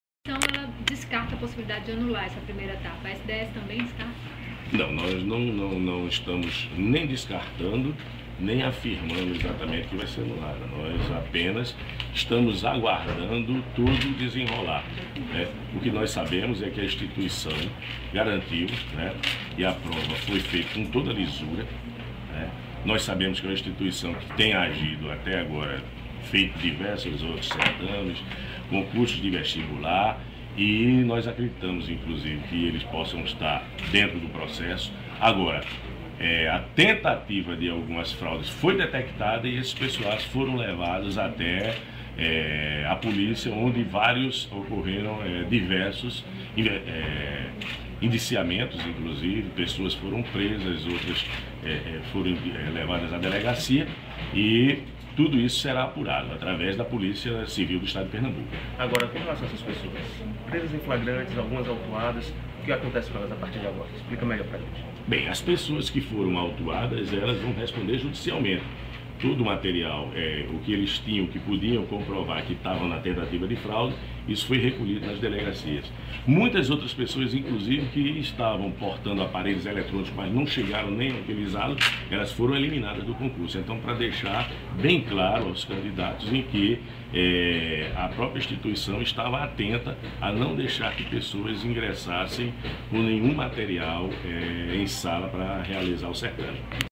Organizadora do concurso da PM nega ocorrência de fraudes
Não houve fraudes, apenas tentativas frustradas. Foi com essa afirmação que o coordenador da Comissão de Concurso da Universidade de Pernambuco (Conupe), ligada ao Instituto de Apoio à Universidade de Pernambuco (Iaupe), Gledston Emereciano, resumiu os problemas ocorridos durante a primeira fase do concurso da Polícia Militar de Pernambuco. Imagens: Aline Moura/DP/D.A Press.